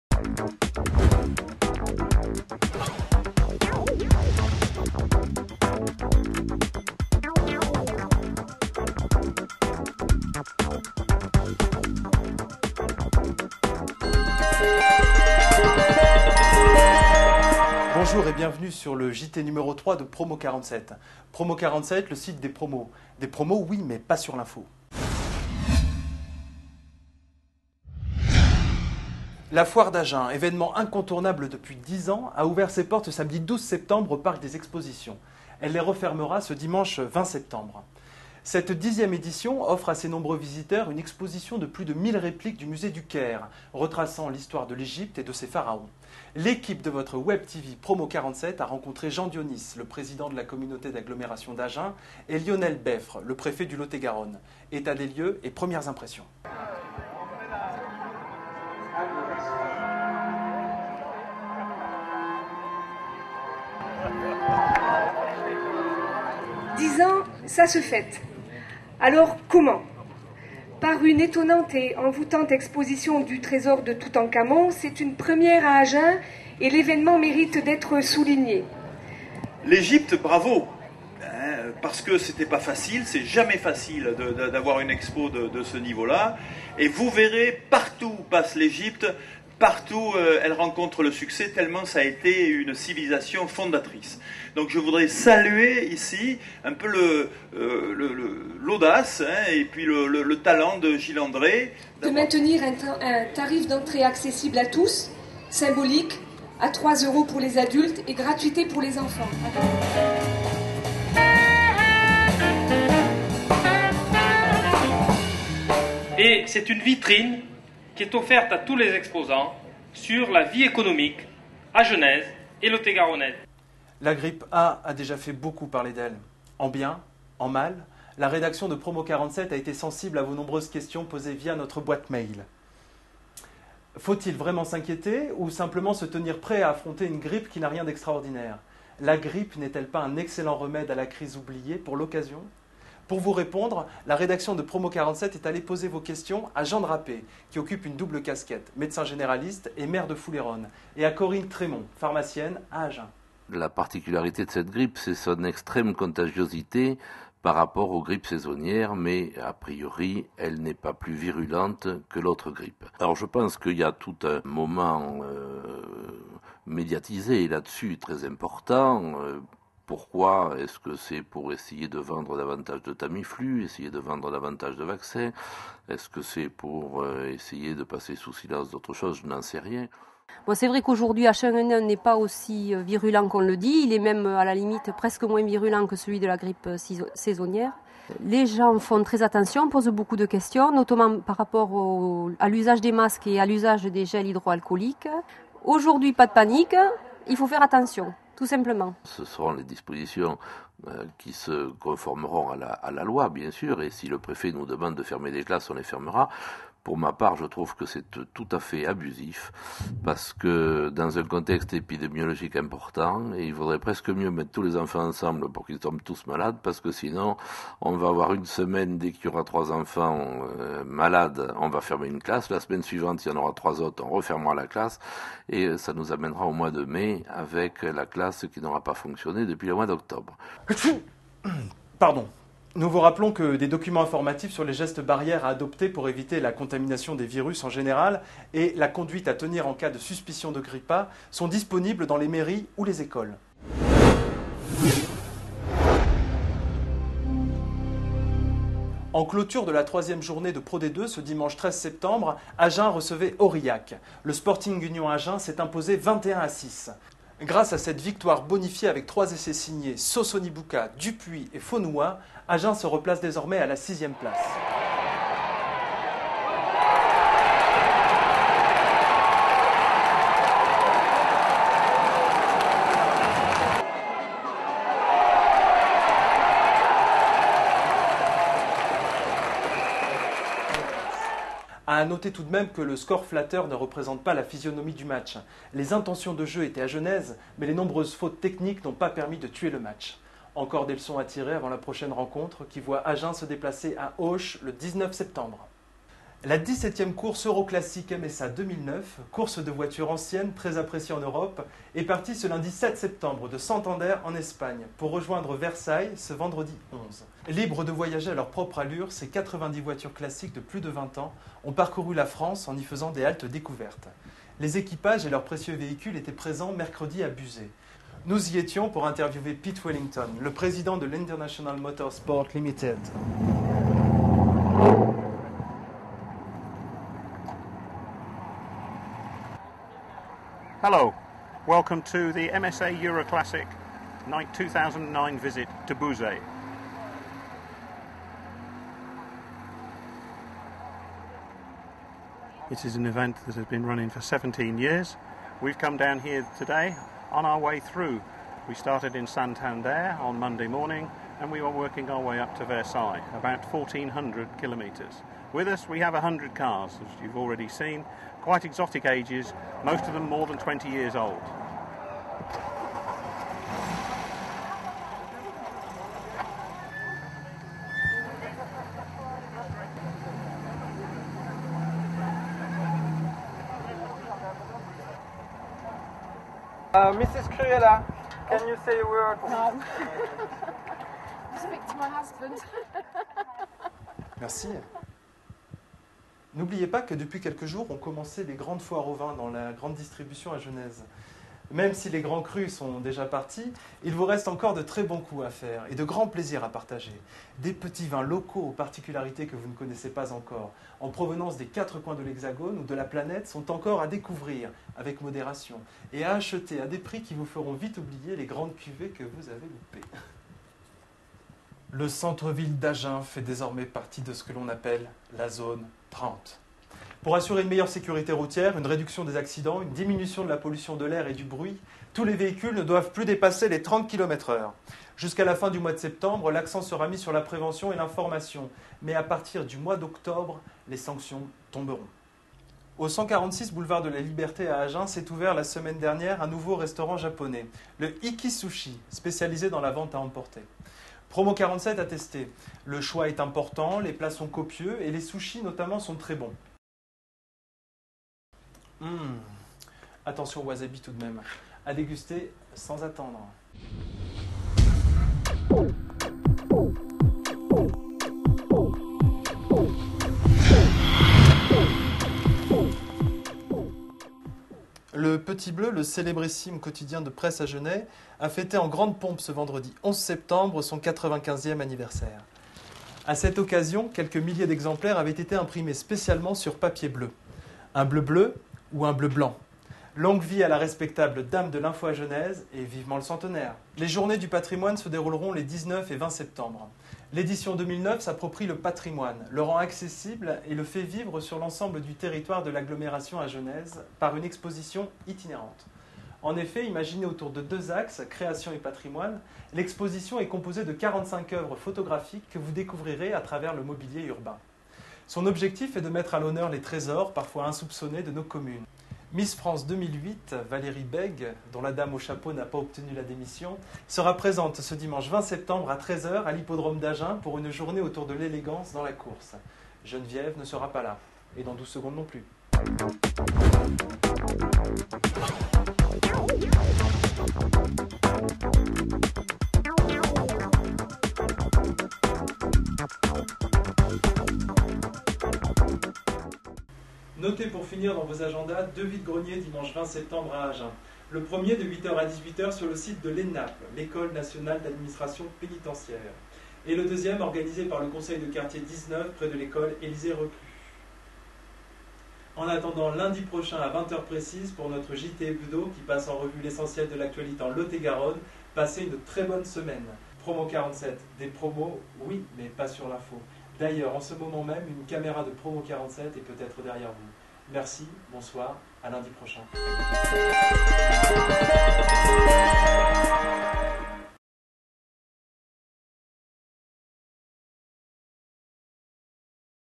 Promo47.fr - JT Foire d'Agen 2009 (Parc des Expos) partie 1
Foire d'Agen 2009 au parc des expositions
1ère partie

Vous allez entendre parler de vous.
Mieux que la Télévision. Votre WebTv locale.
Des promos oui mais pas sur l'info !